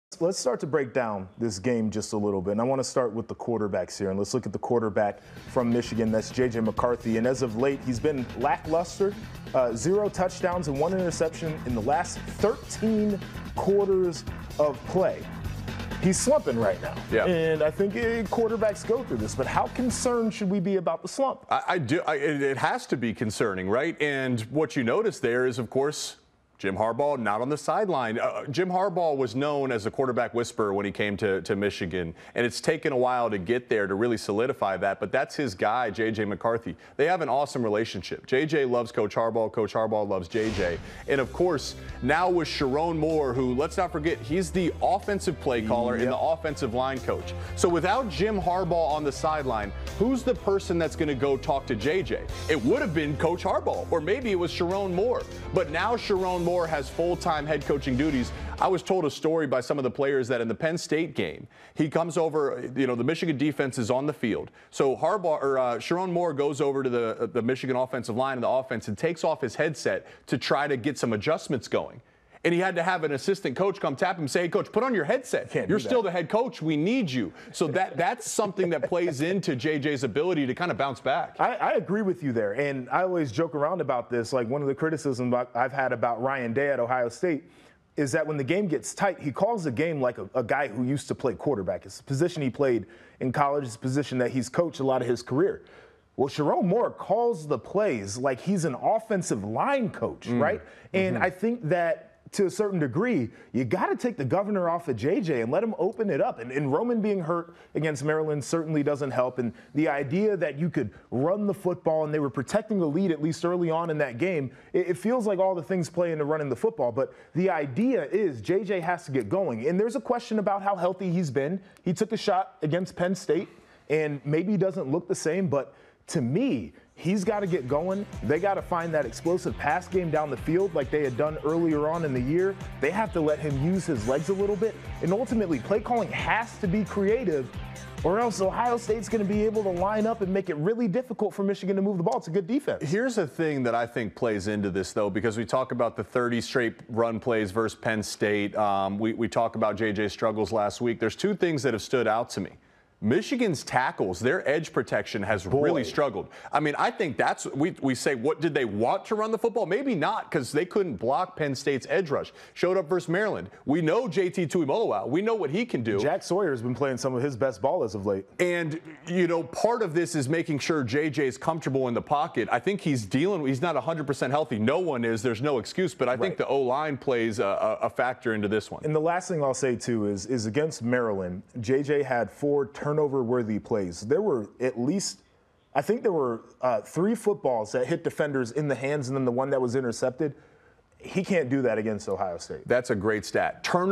J.J. McCarthy Has Had Some Recent Struggles for Michigan | Red Corner Blue Corner
J.J. McCarthy has struggled recently for Michigan.